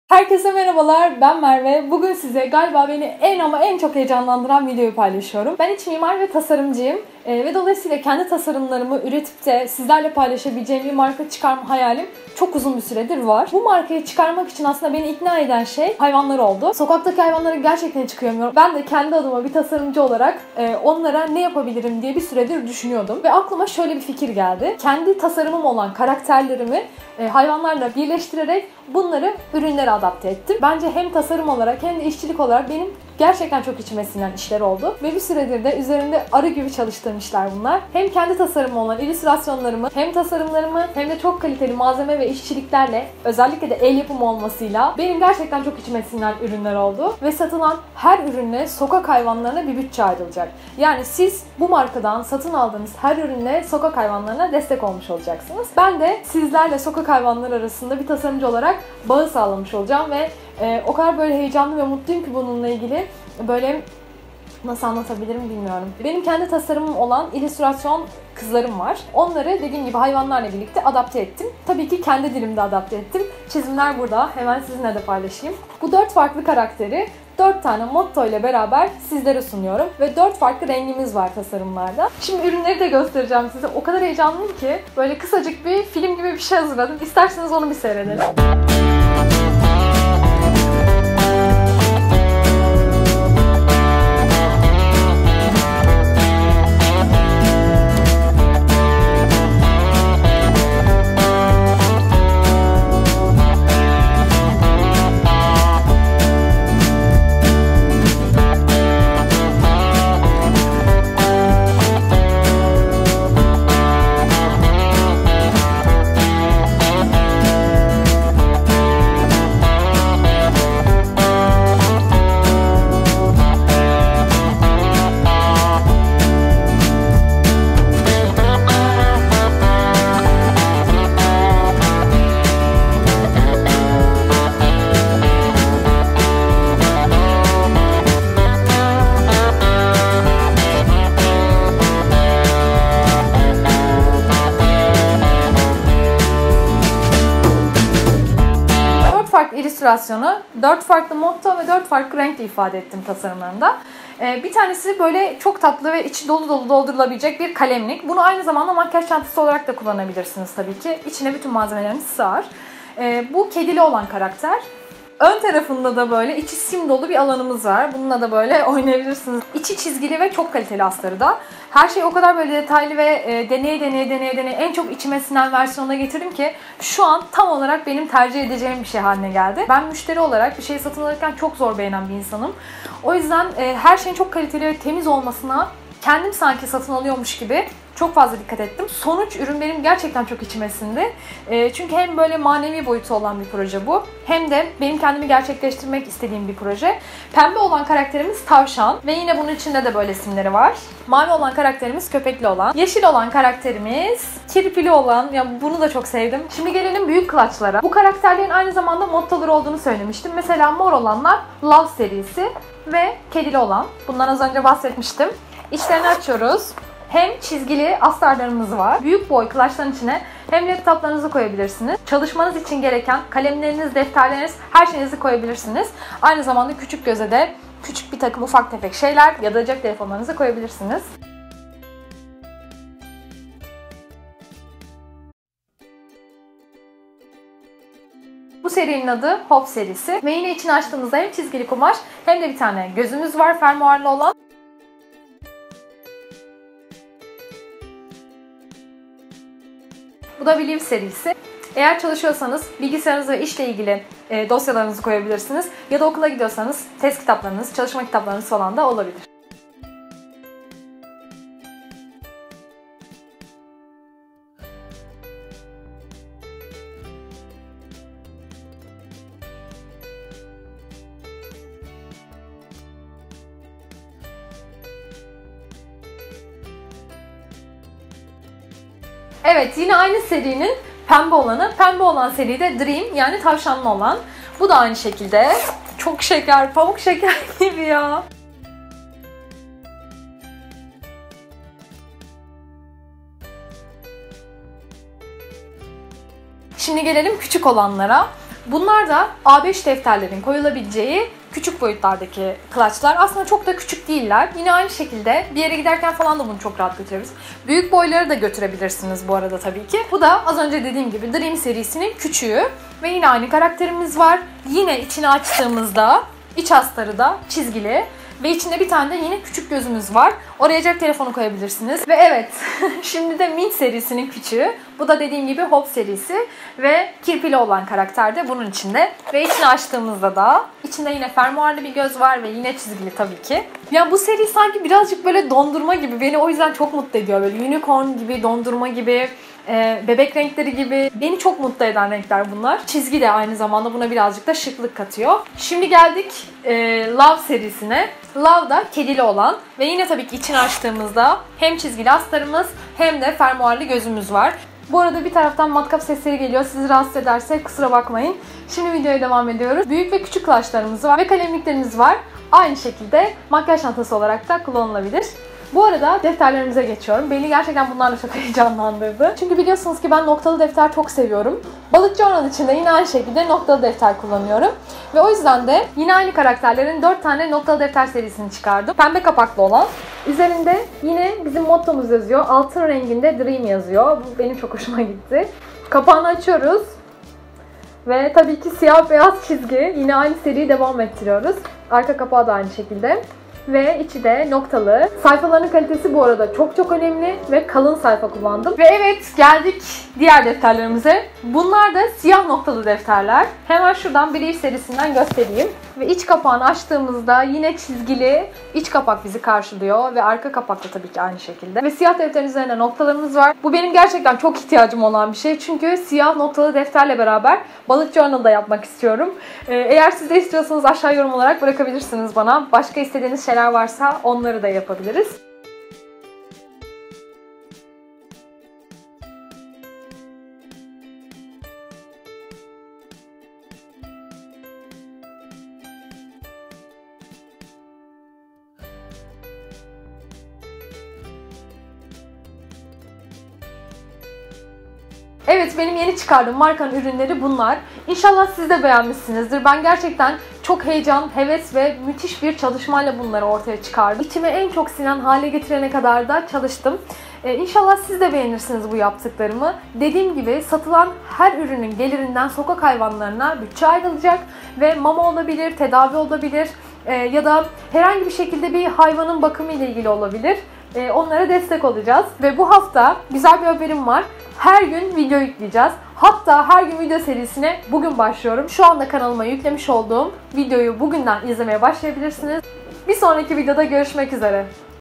KENDİ MARKAMI ÇIKARDIM I Sokak Hayvanlarına Destek Oluyoruz
Herkese merhaba bugün sizlere heyecanla üzerinde çalıştığım kendi tasarımım olan kırtasiye ürünlerimi paylaşacağım. Sanırım en heyecanlı olduğum video :) Satılan bütün ürünlerin gelirinden sokak hayvanlarına bütçe ayrılacak, bir tasarımcı olarak sizlerle sokak hayvanları arasında böyle bir bağ kurmak istedim. Güzel yorumlarınızı ve desteğinizi bekliyorum.
Hepinizi seviyorum..